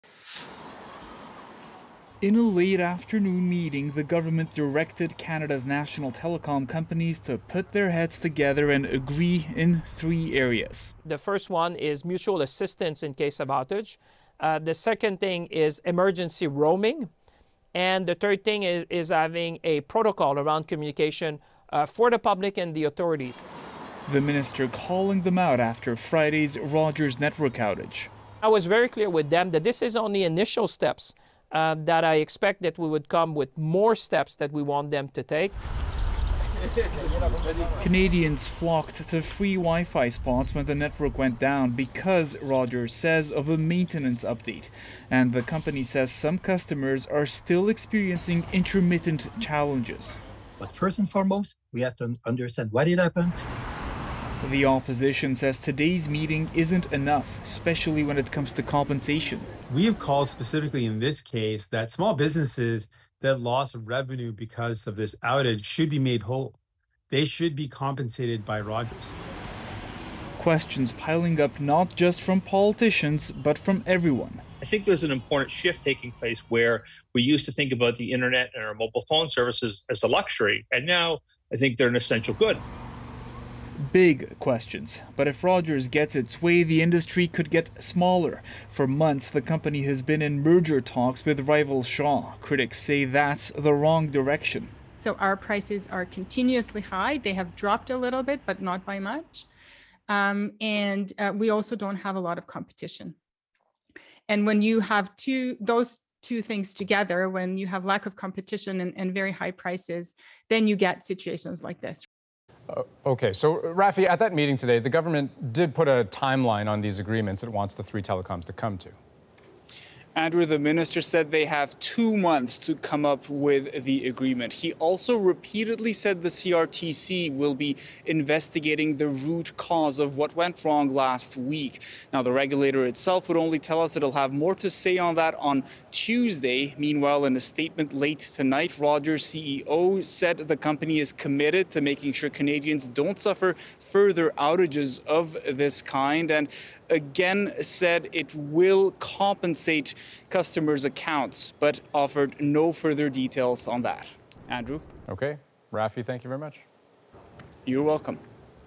Ottawa demands telecoms create backup plan for network outages
The federal government is giving the big three telecommunications companies 60 days to agree on a plan for assistance during service outages; emergency roaming; and communication about major disruptions.